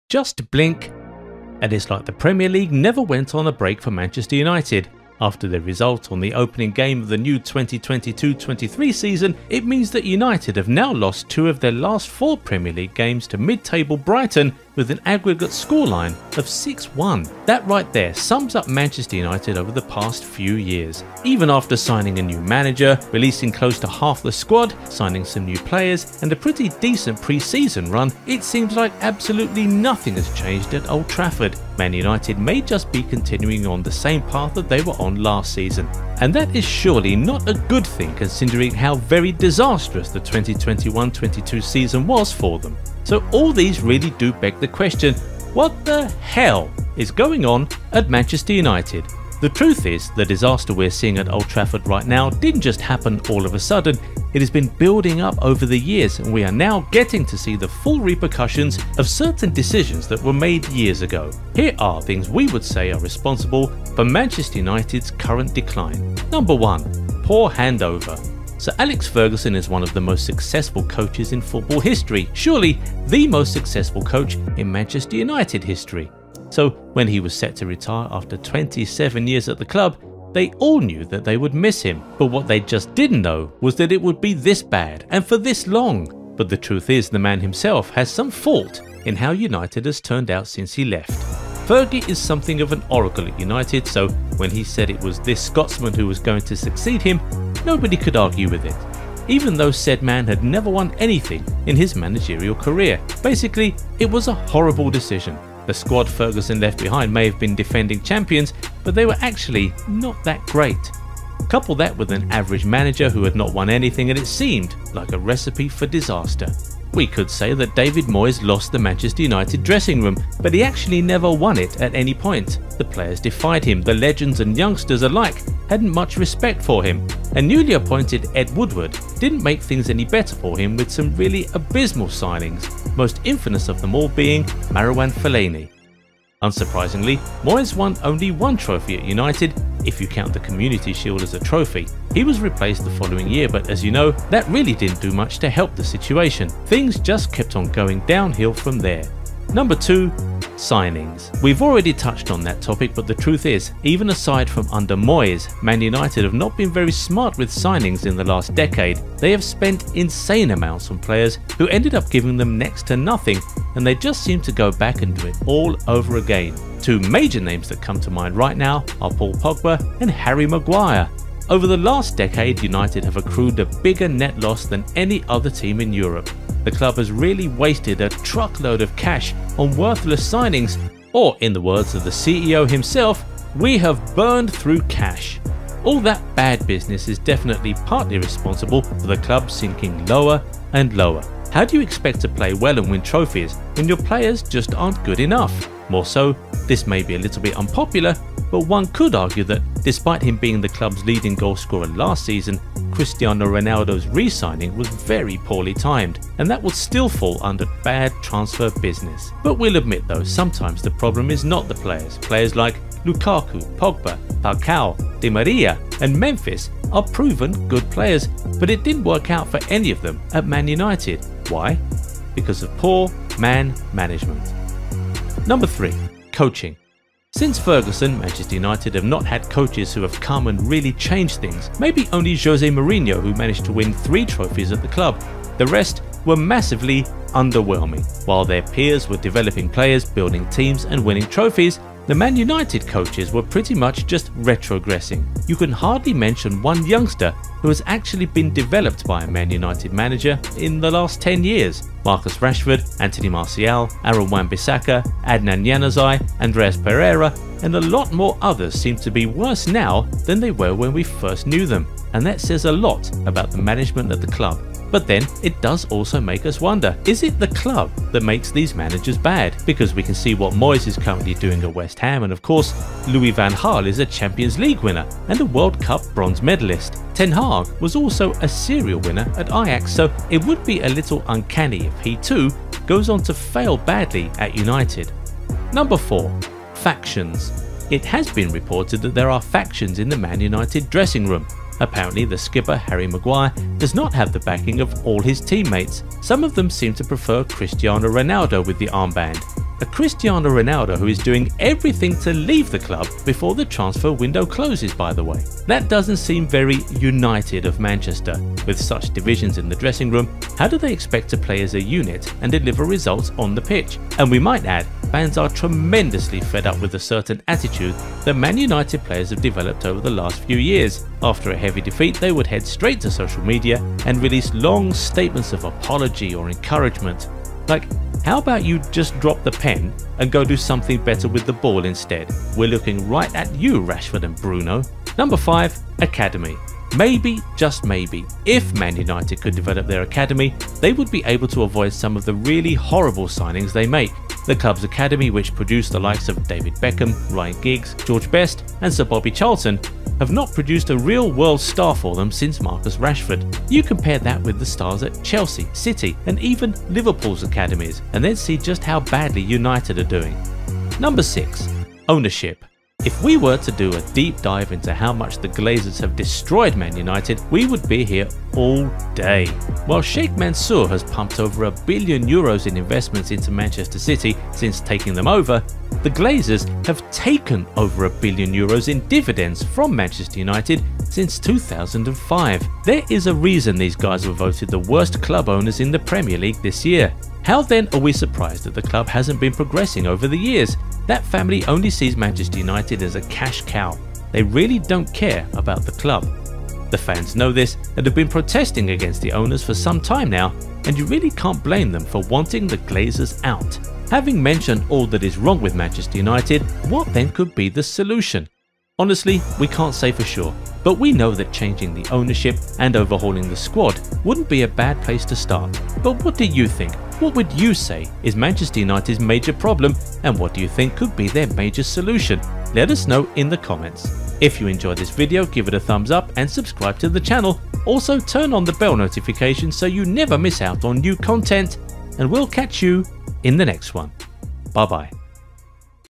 What The Hell Is Going On At Manchester United? (2022)
Just blink and it’s like the Premier League never went on a break for Manchester United. After their result at the opening game of the new 2022-23 season, it means that United have now lost 2 of their last 4 Premier League games to mid-table Brighton with an aggregate scoreline of 6-1. That right there sums up Manchester United over the past few years.

So, even after signing a new manager, releasing close to half of the squad, signing new players, and a pretty decent preseason run, it seems like absolutely nothing might have changed at Old Trafford. Man United may just be continuing on the same path that they were on last season. And that is surely not a good thing considering how very disastrous the 2021-22 season was for them.

So, all these really beg the question; what the hell is going on at Manchester United?

The truth is, the disaster we are seeing at Old Trafford right now did not just happen all of a sudden. It has been building up over the years and we are now getting to see the full repercussions of certain decisions that were made years ago.